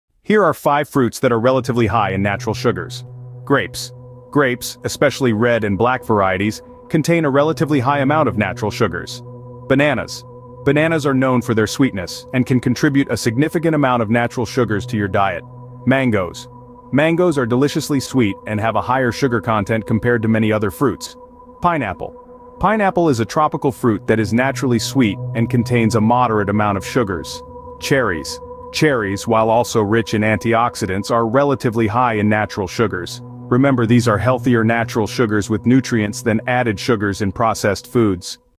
5 Fruits High in Sugar
Dive into the world of sweet fruits! Discover the natural sugars in grapes, bananas, mangoes, pineapple, and cherries. Learn about healthy snacking and embrace the goodness of these delicious treats!